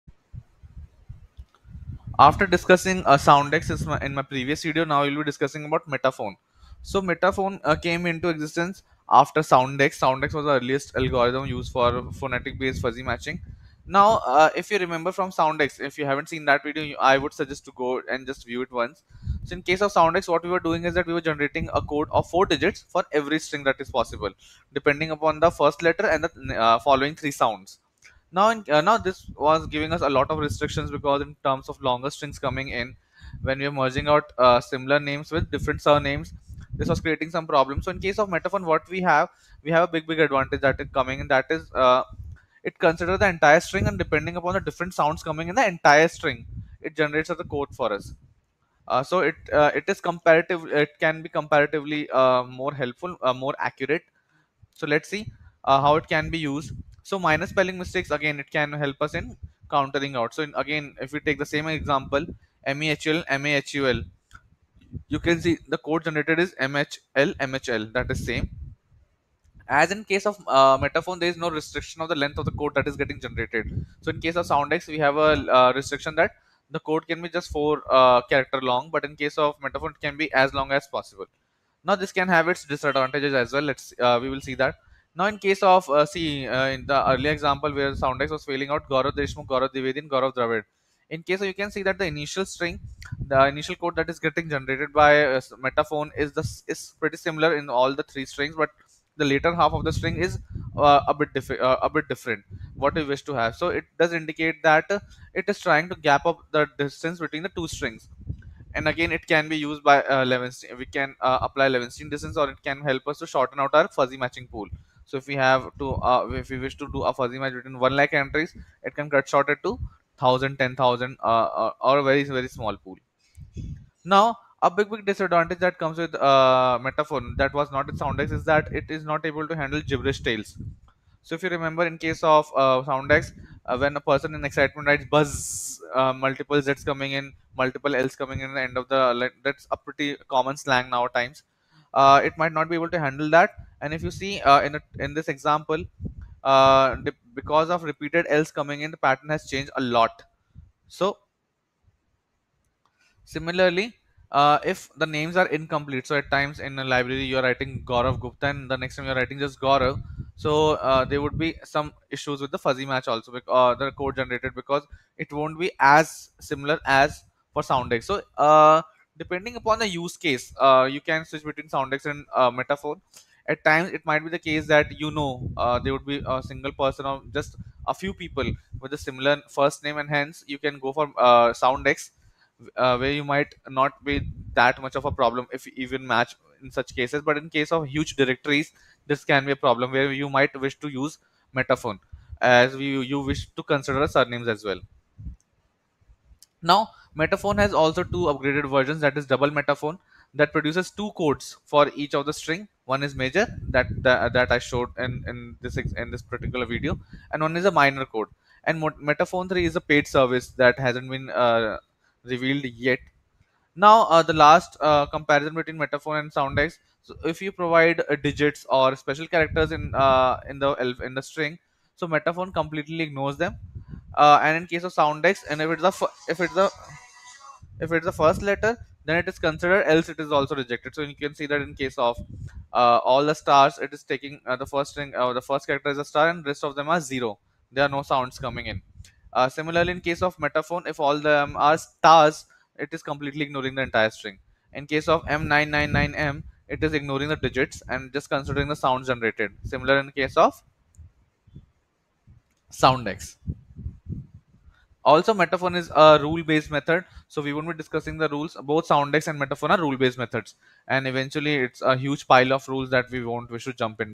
Metaphone fuzzy matching algorithm
A sound based rule based fuzzy match algorithm used usually for names matching